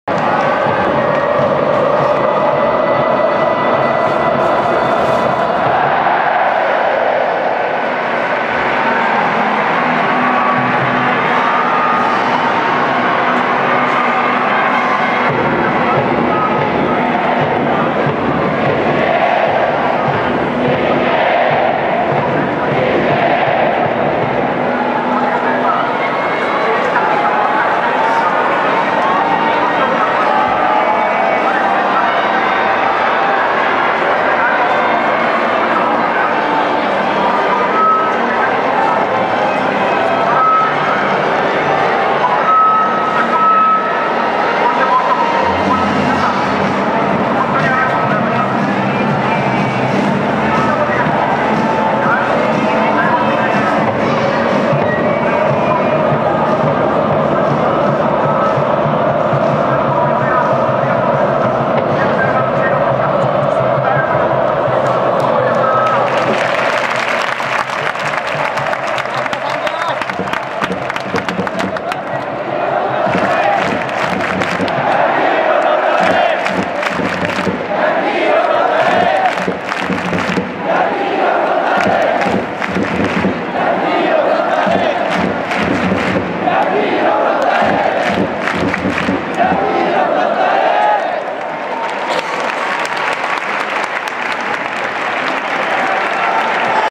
【川崎フロンターレ】2017年1月1日 風間八宏監督挨拶
第96回天皇杯全日本サッカー選手権大会 決勝 vs鹿島アントラーズ（@市立吹田サッカースタジアム）